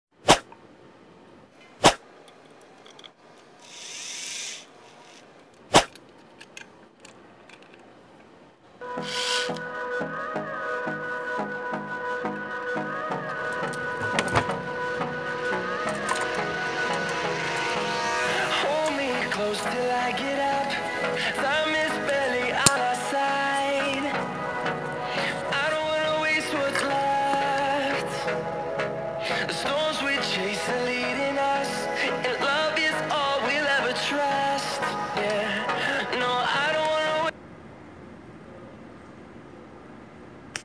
SIMPLE TOOL TIPS AND TRICKS INVENTION: Fastened Chain Easily
How to fastened the chain easily! Tips and tricks ideas you must know. Tools invention for you! Tutorial videos for beginners.